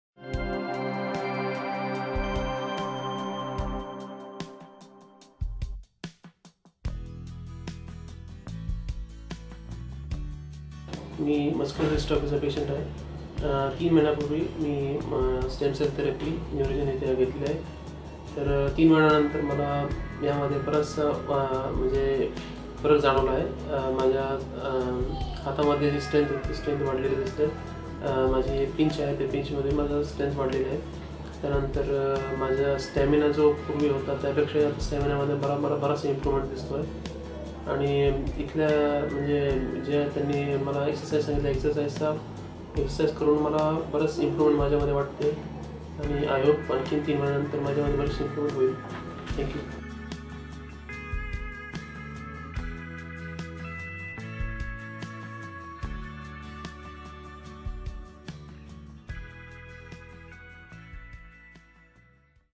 Limb Girdle Muscular Dystrophy Treatment | Results after 3 months
His pinch strength and stamina improved after Limb Girdle Muscular Dystrophy Treatment.